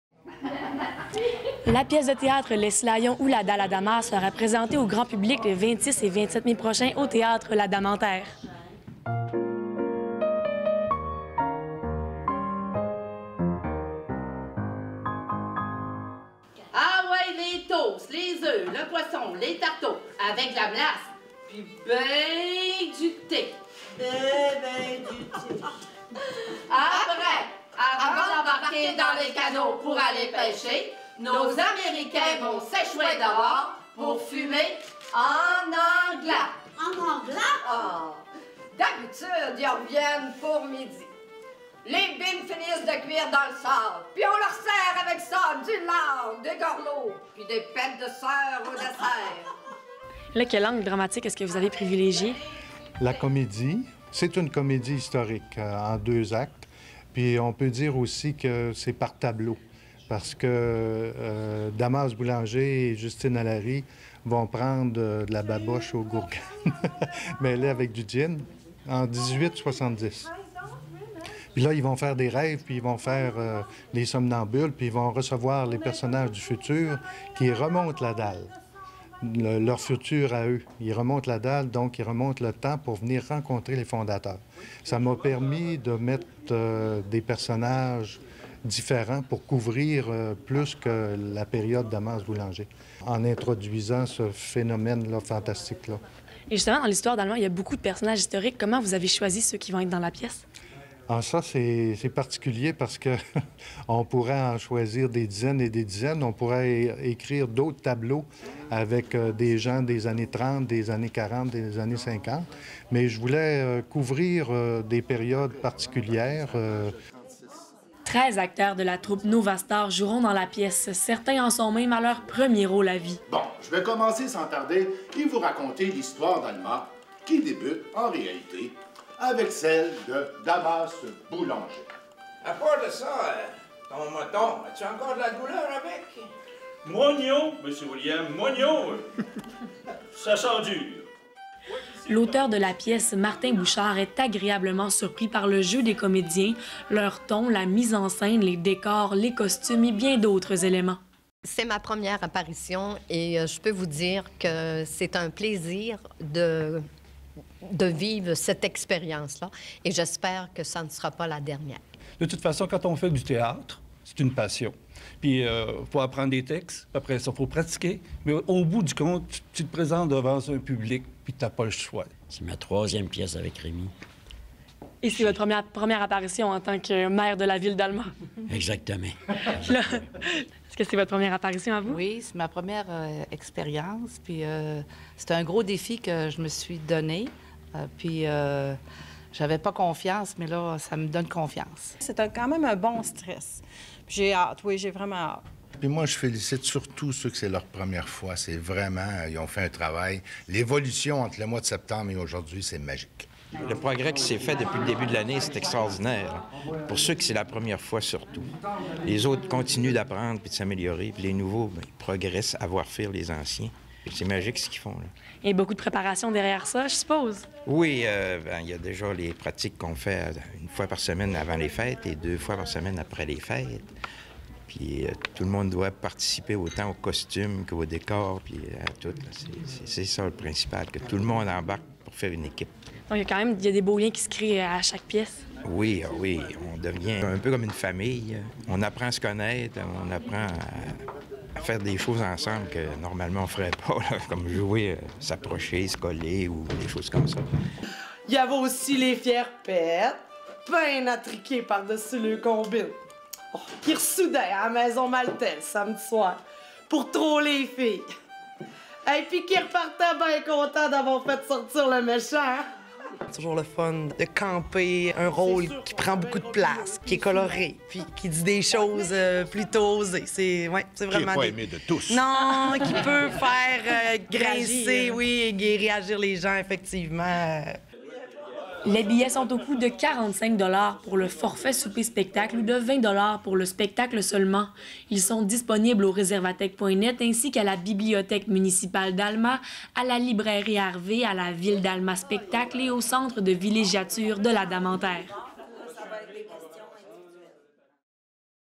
NovaStar
La troupe de théâtre NovaStar présente la pièce Les Slaillons ou La dalle à Damase.